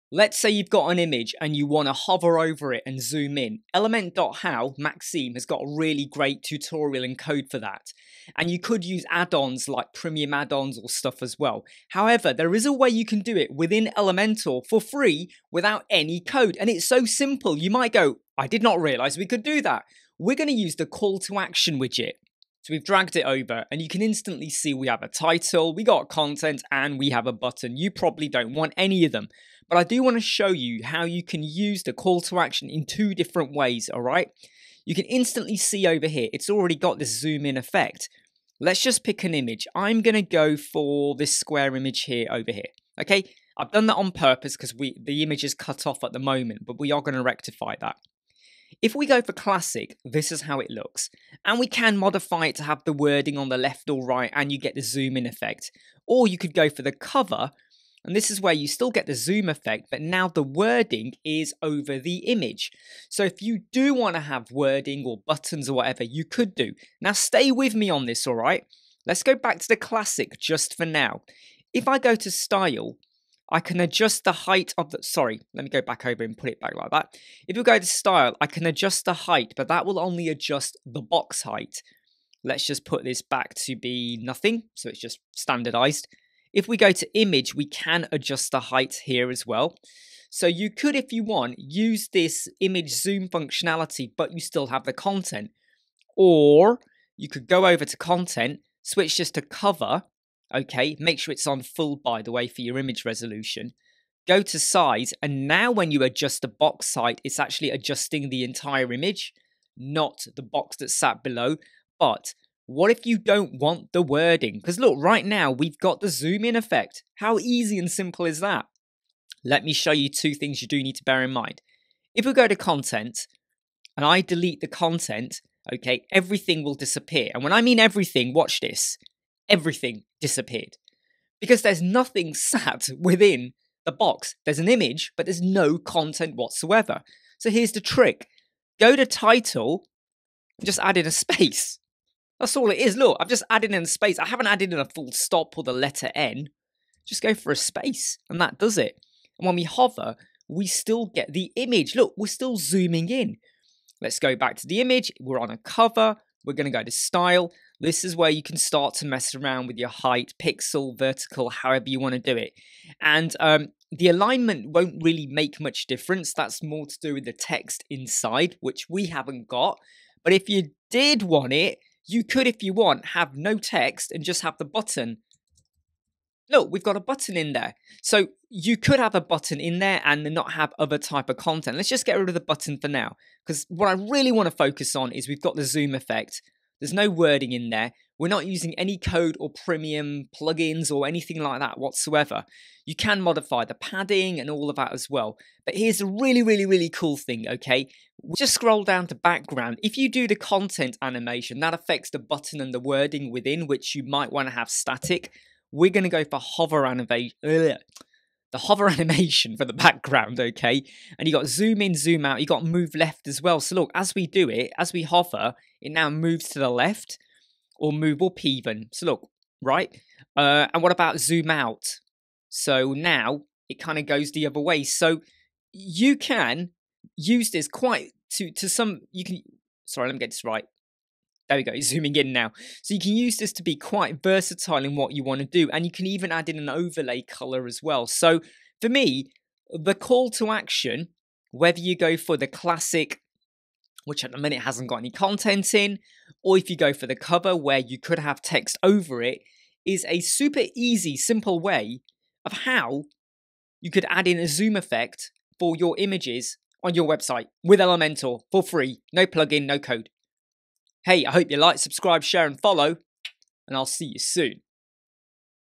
Elementor Wordpress Tutorial - Image Zoom with No Code and No Plugin
Without using any Plugins or Code - we can use a Native Elementor Widget to apply some Image Zoom - with wording over it - if you so wish :)

So simple :)

3m Ethernet Cat 7 Cable (to maximise the Internet Connection as the room is upstairs)